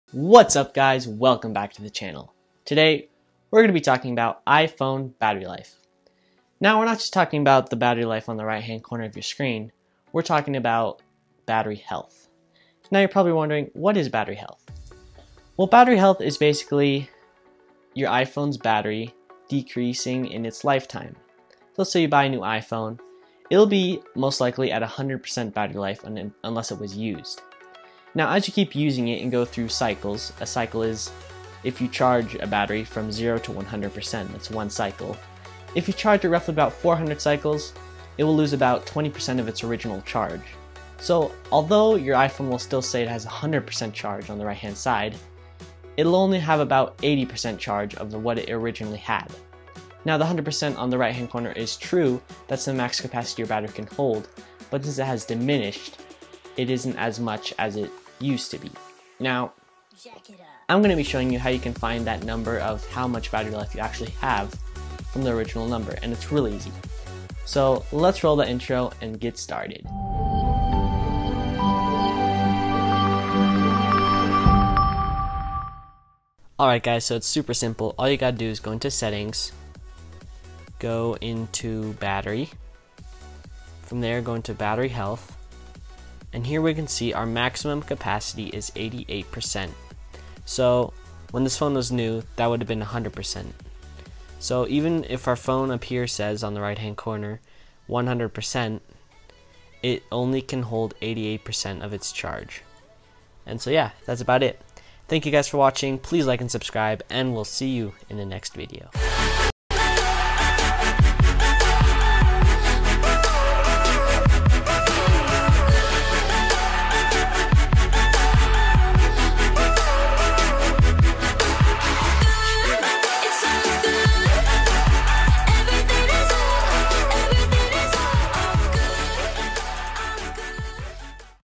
how to check iPhone battery health IOS 12
In this video, I will explain iPhone battery life and show you how to find it in your settings.